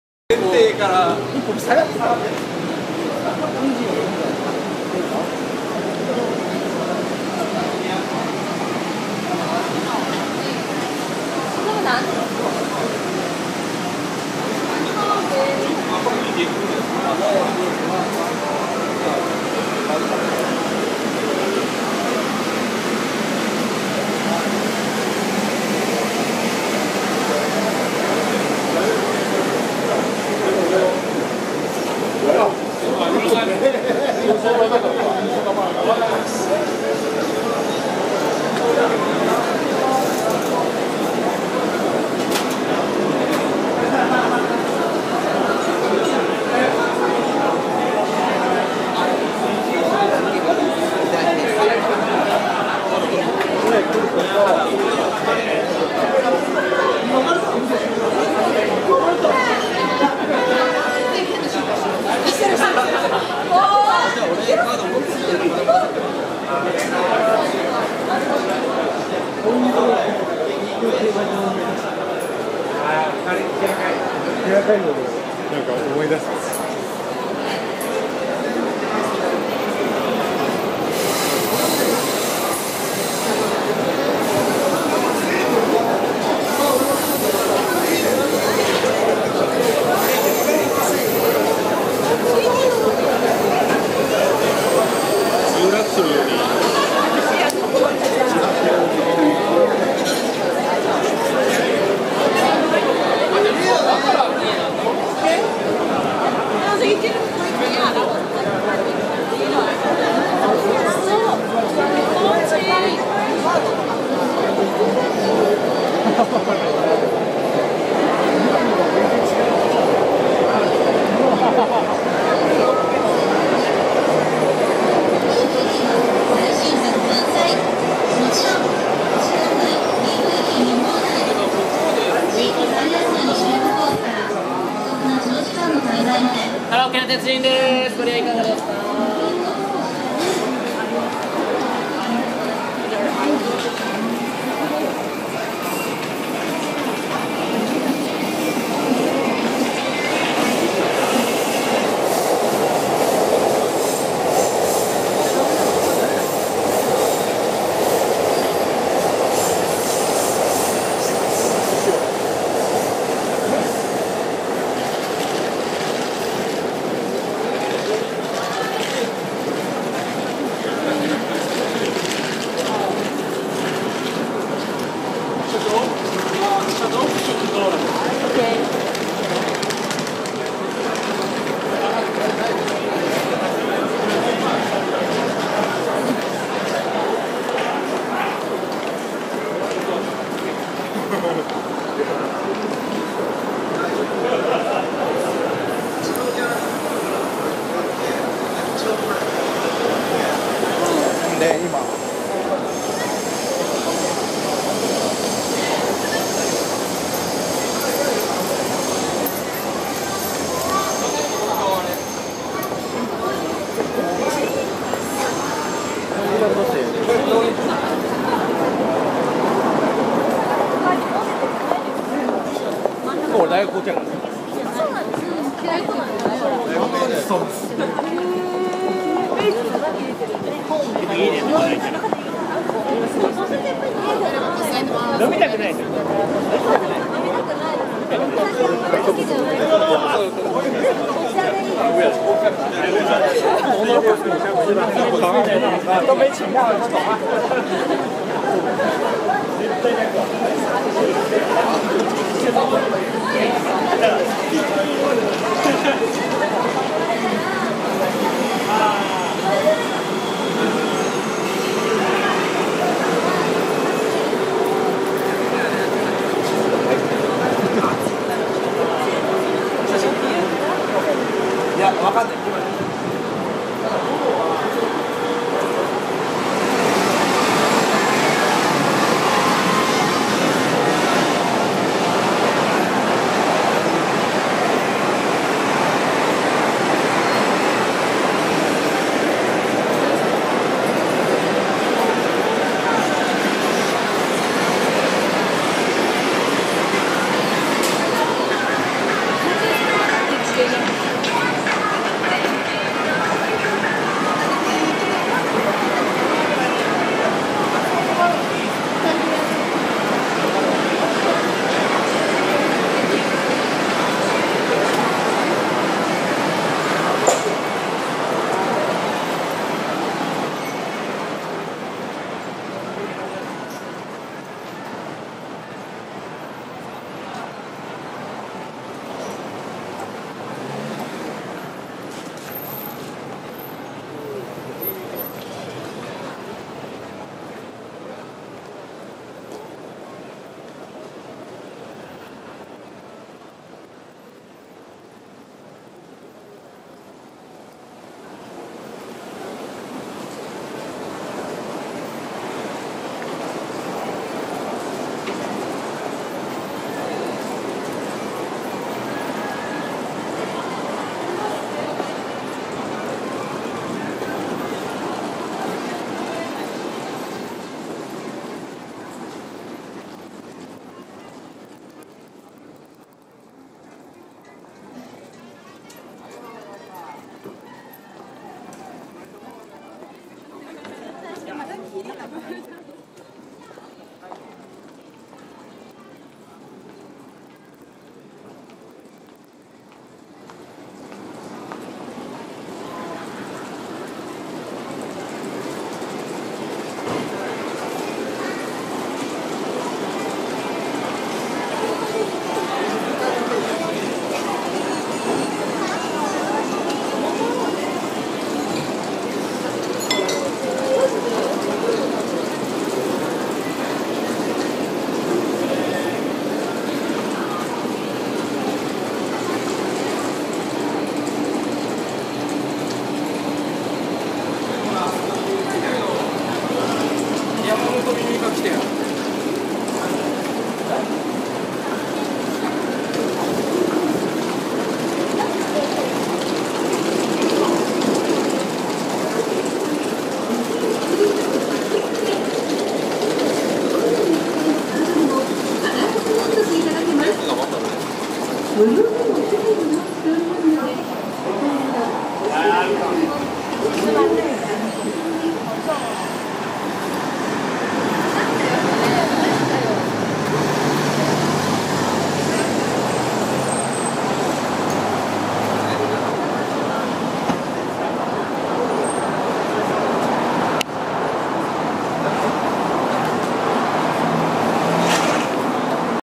Un giro fra i locali di Ameyokocho
Quando arriva la sera, subito dopo l'orario d'ufficio, nelle viette di Ameyoko cho, di fronte all'uscita centrale della stazione di Ueno, tutti si riversano negli izakaya e nei localini sotto i binari della ferrovia per passare momenti di relax in compagnia e ubriacarsi per dimenticare la giornata appena passata. Uno dei modi migliori per respirare la vera atmosfera di Tokyo.